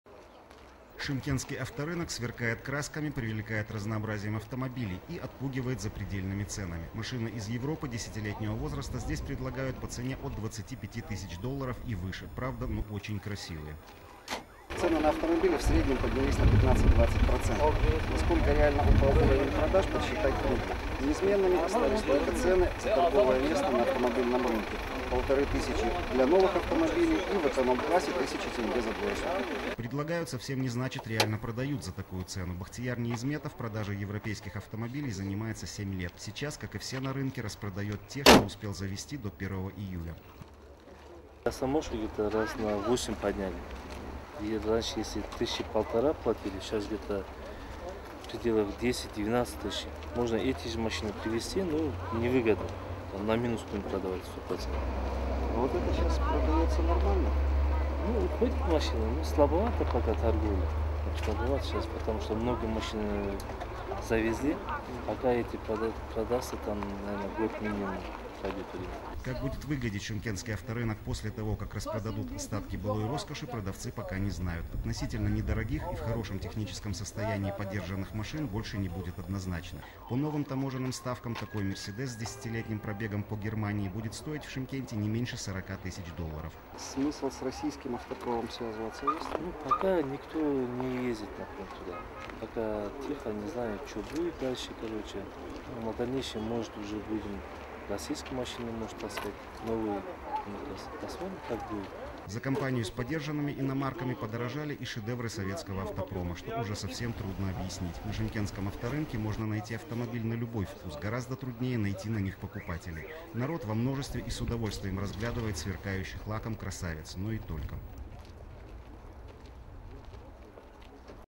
Стоимость подержанных автомобилей
Стоимость поддержанных машин выросла на 15-20%. И это притом, что нового завоза не было, автоперевозчики распродают то, что успели ввести до 1 июля 2011 года.